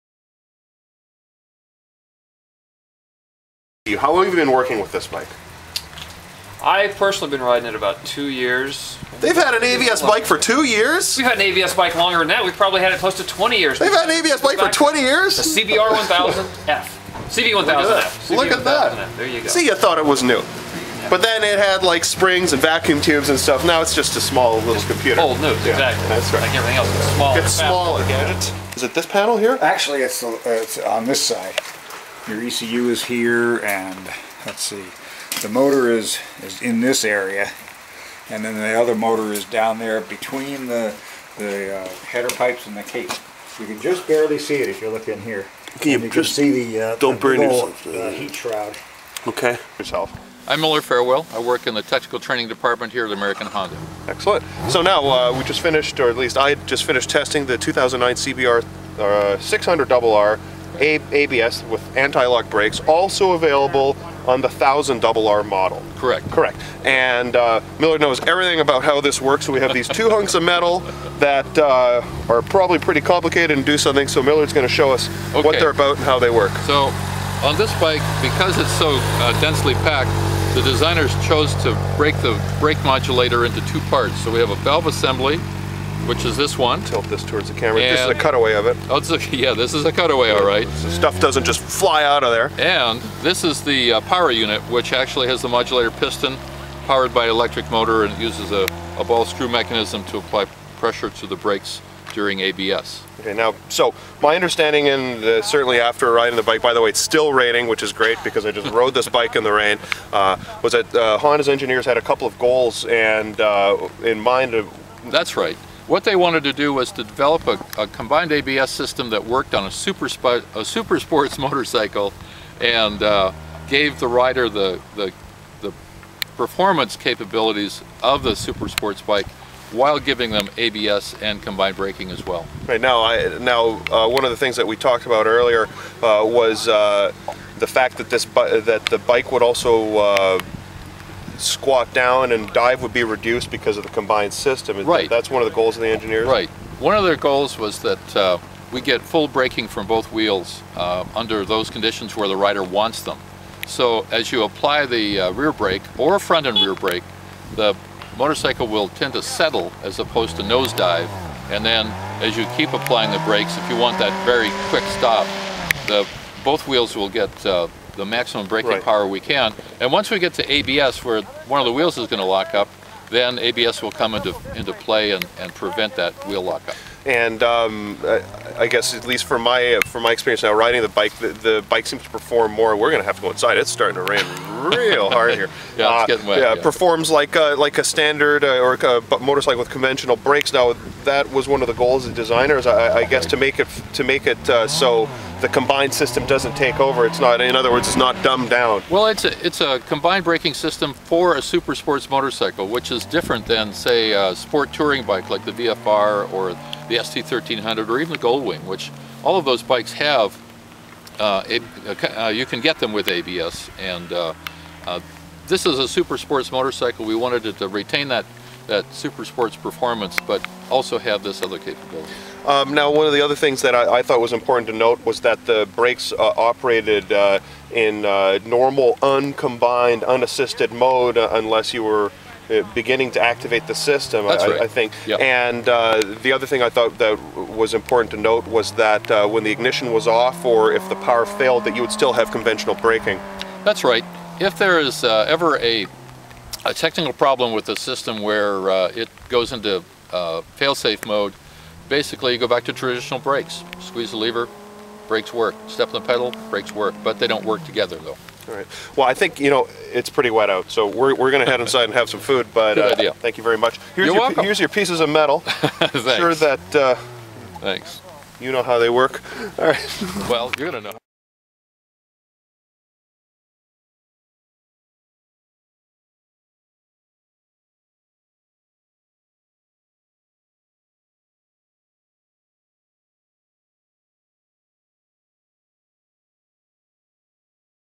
2009 Honda Combined ABS System Explained
Honda's new SuperSport Combined Anti-Lock Braking System explained in detail at the North American Press Launch for the 2009 CBR600RR ABS.